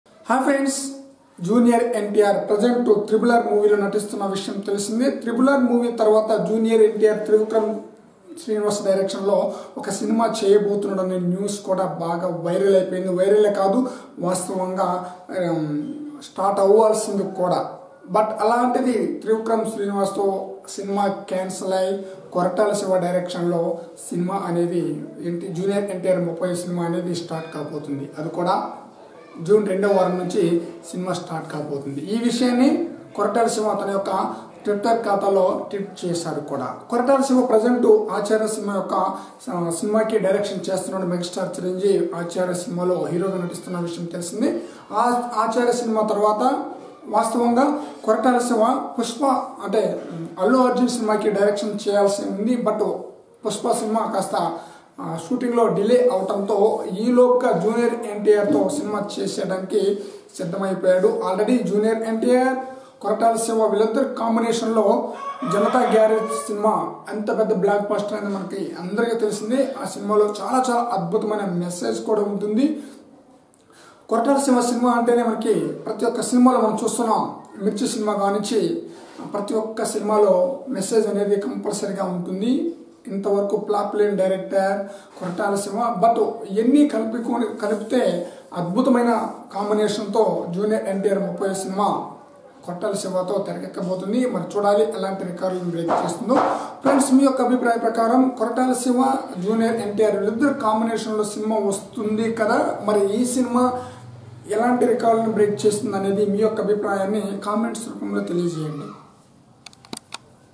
NTR 30 Movie Updates | Jr NTR New Movie | Koratala Siva | RK Channel
ntr prastutam rrr movielo natistunnadu,rrr tarwata trivikram srinivas directionlo cinema utundi anukunnaru andaru,kani NTR30 cinema KoratalaSiva directionlo rabotundi,already veeriddari combolo janata garage movie super hit cinema vachindi,malli ee combo meeda anchanalu bhari stayilo undi,